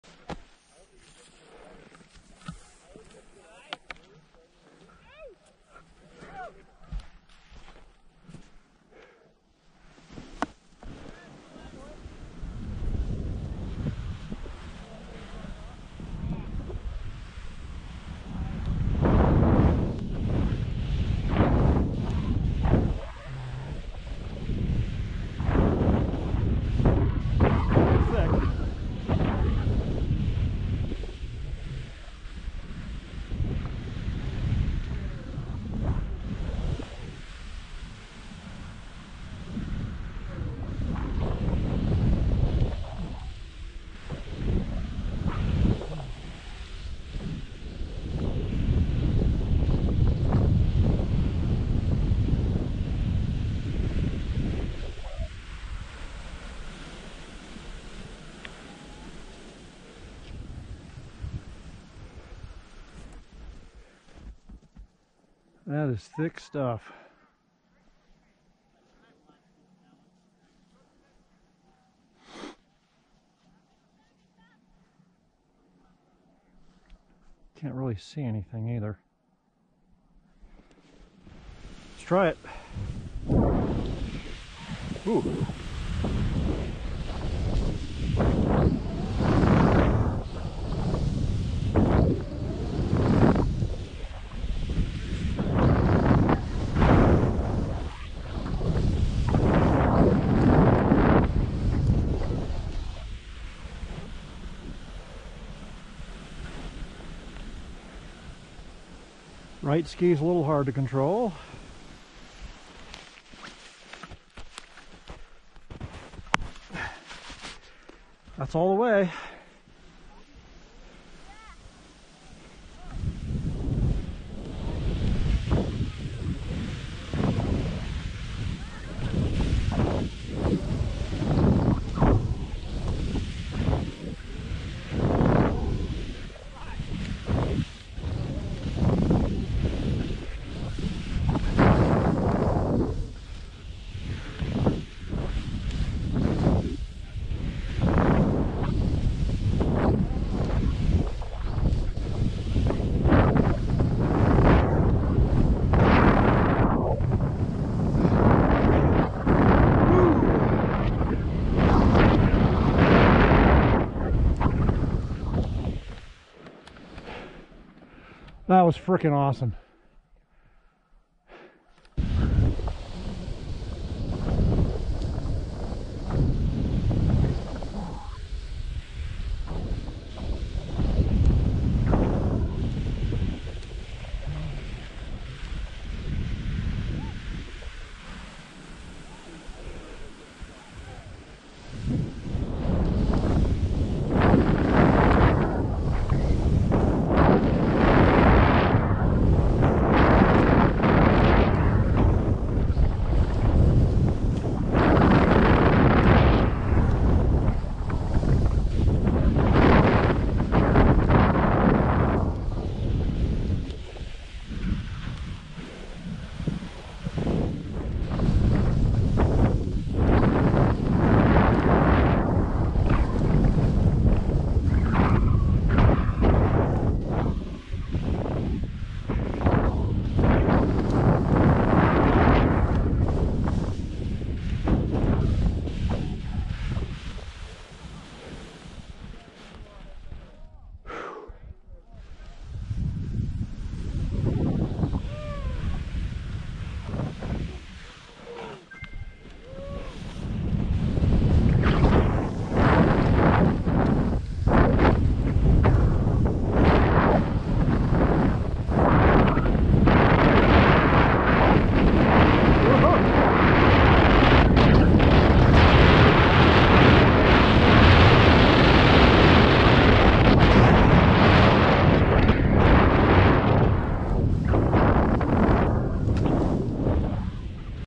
Devils Castle Jan 26, 2020
Alta Ski Area
Not great lighting today and the snow was heavy.  But still very fun to ski.